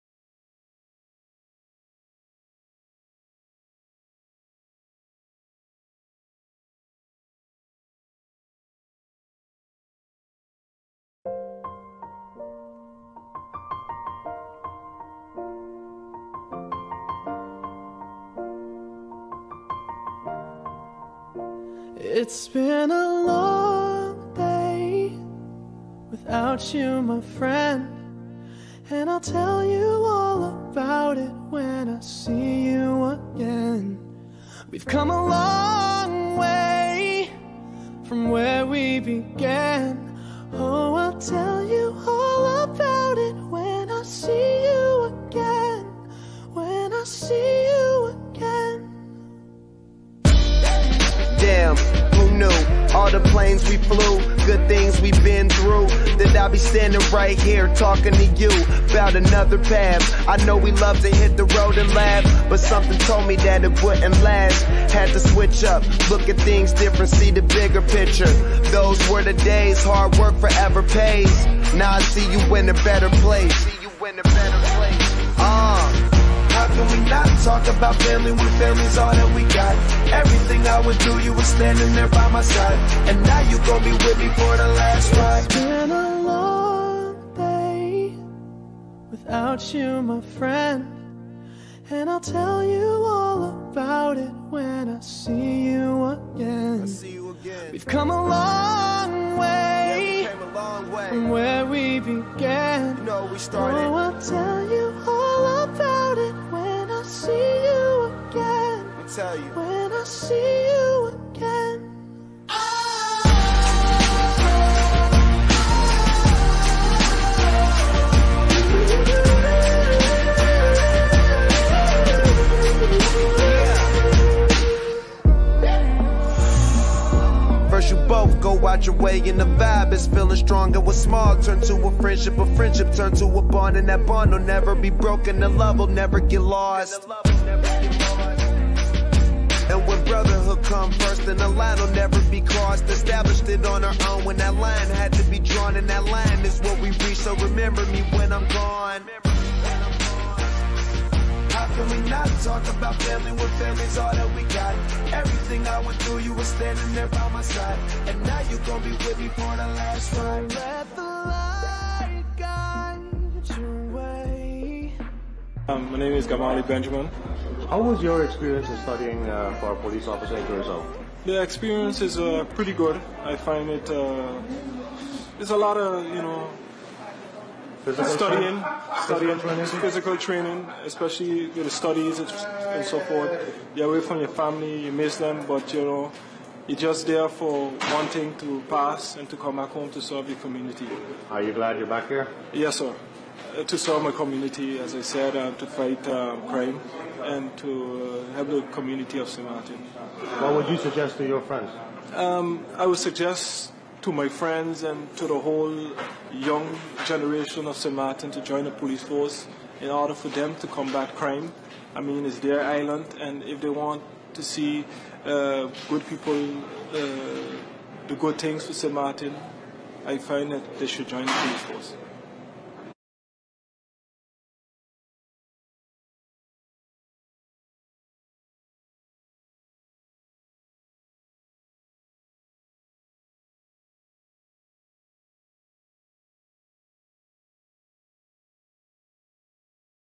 Remembering Officer Gamali Benjamin
Remembering officer Gamali Benjamin who passed away tragically on August 8th 2015 on Sint Maarten. He was a member of the Sint Maarten Police Force. Officer Benjamin was shot while responding to an armed robbery on a jewelry store in Philipsburg, Sint Maarten. Continue to sleep in peace